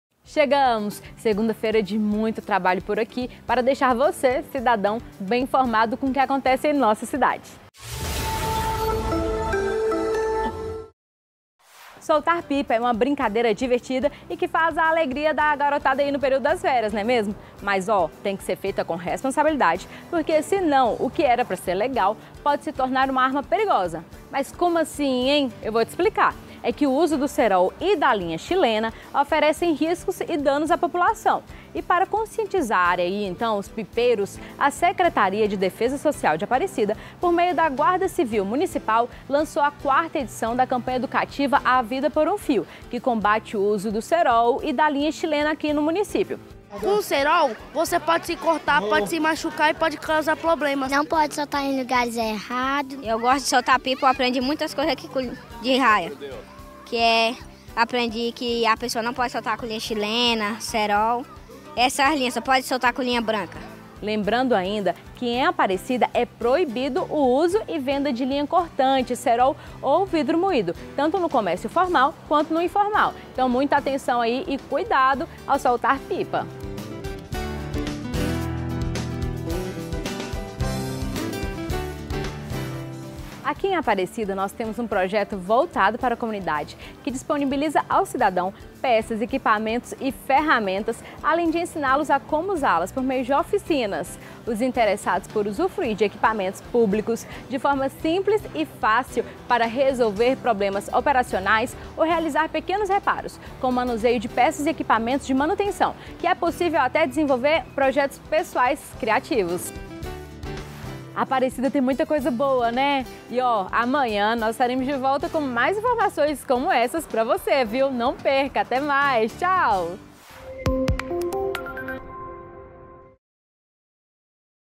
4ª edição da campanha educativa A vida por um fio
A Secretaria de defesa social de Aparecida por meio da Guarda Civil Municipal lançaram a 4ª edição da campanha educativa A vida por um fio, que combate o uso do cerol e a linha chilena no Município.